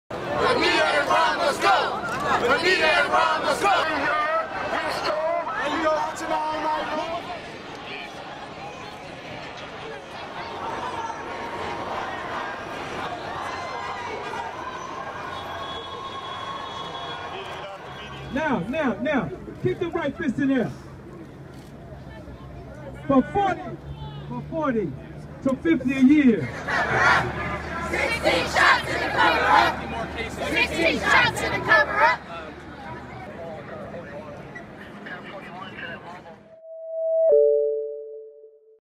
Chicago Protesters Call For Mayor's Resignation
Chicago Mayor Rahm Emanuel apologized for the 2014 police shooting death of Laquan McDonald, but protesters still want his resignation.

In a rally on Wednesday, demonstrators called for Cook Count State's Attorney Anita Alvarez, who has come under intense scrutiny for her office's role in the investigation, to step down as well.

Protesters have turned out en masse since the release of the video showing former Chicago Police officer Jason Van Dyke shooting McDonald 16 times, several of those while McDonald was on the ground.